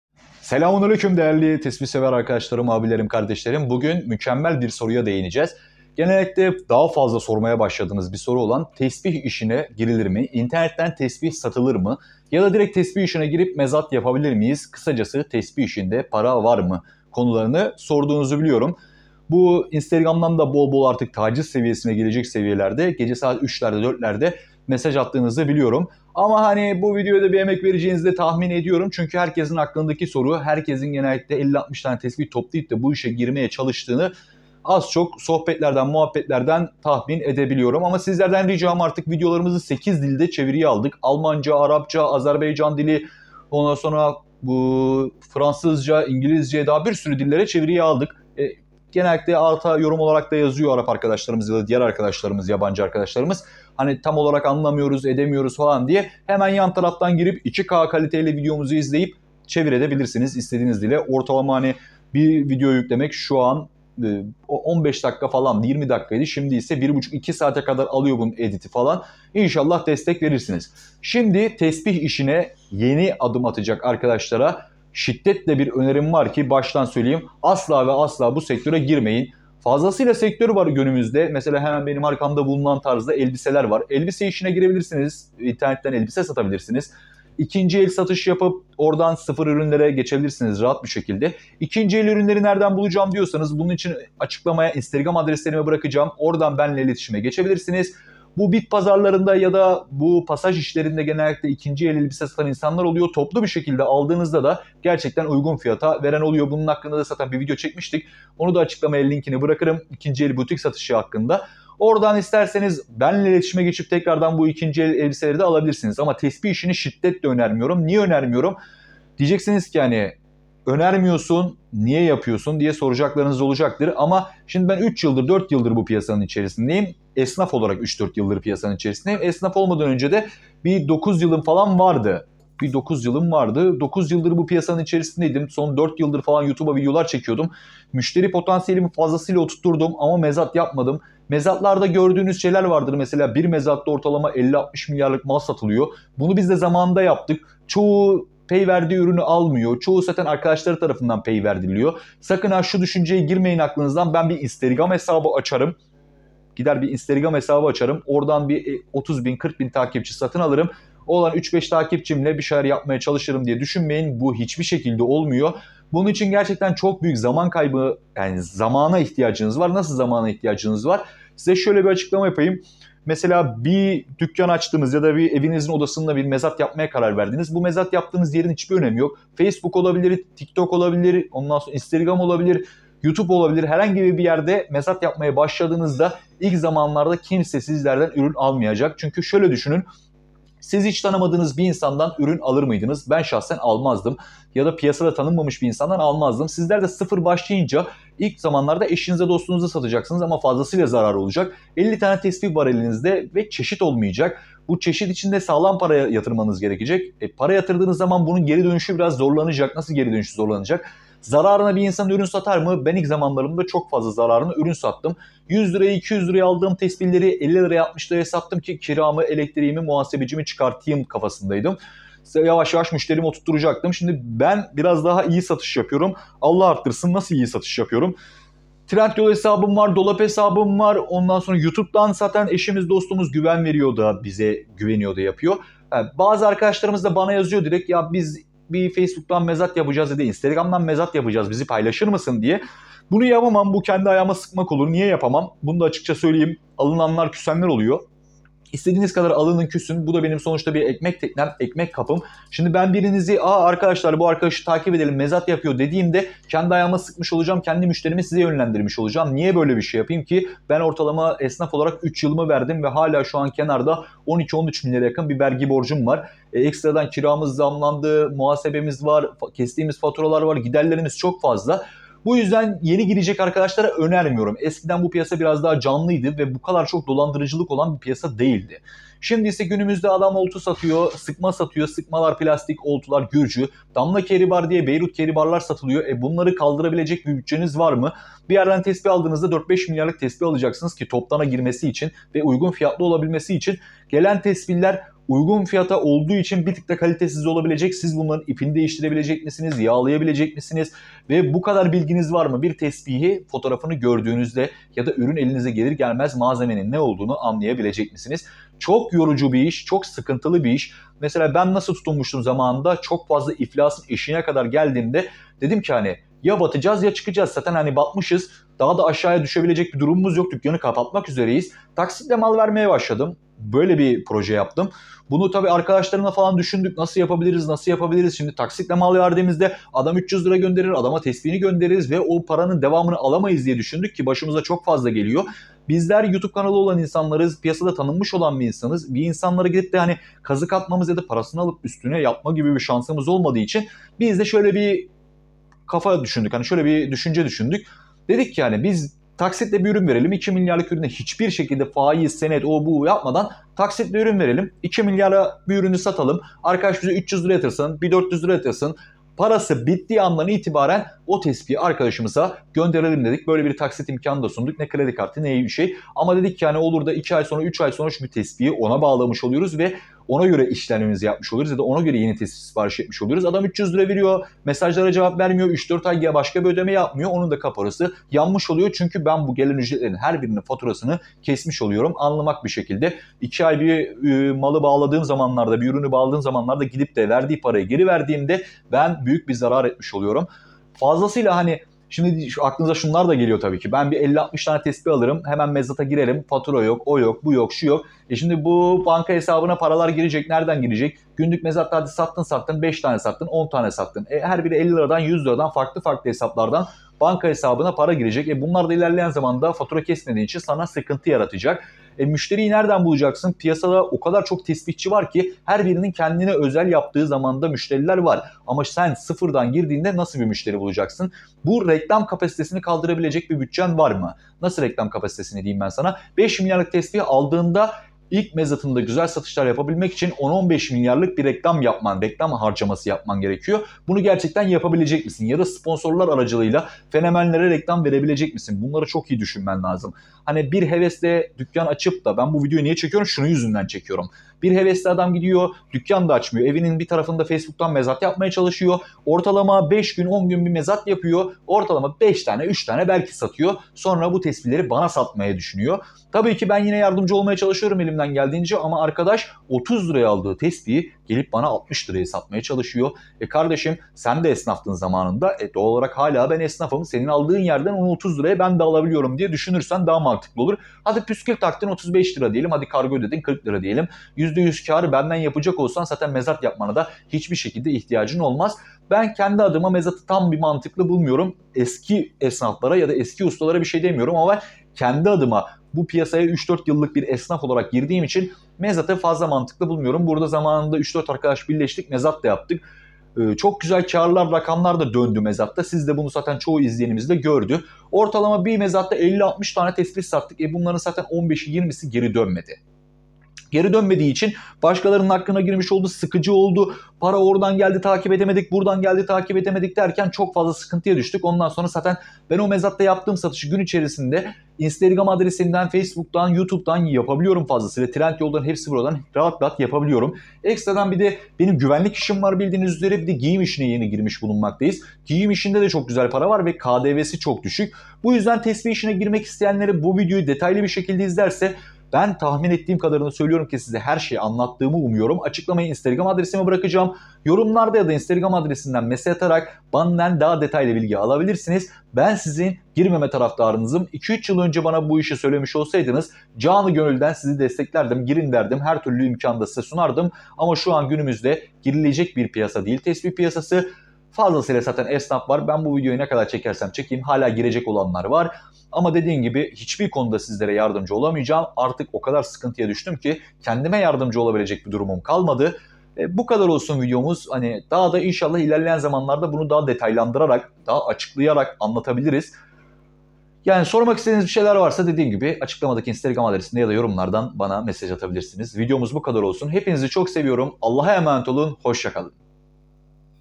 İnternetten Tesbih Satılır mı? Tesbih İşinde Para Varmı?
İnternetten Tesbih Satılır mı? Tesbih İşinde Para Varmı? İnternetten mezat açarak yada sayfa açarak satış yapabilirmiyiz diye fazlası ile soru soruluyor bana bende fazla yogunluğum olduğu için cevap veremiyordum.Güzel bir video çekip size cevap vereyim dedim inşallah hoşunuza gider.Keyifli izlemeler diliyorum.Bana ulaşmak isterseniz instagram adreslerimi sizlere bırakıyorum.
adreslerimden bana ulaşabilirsiniz.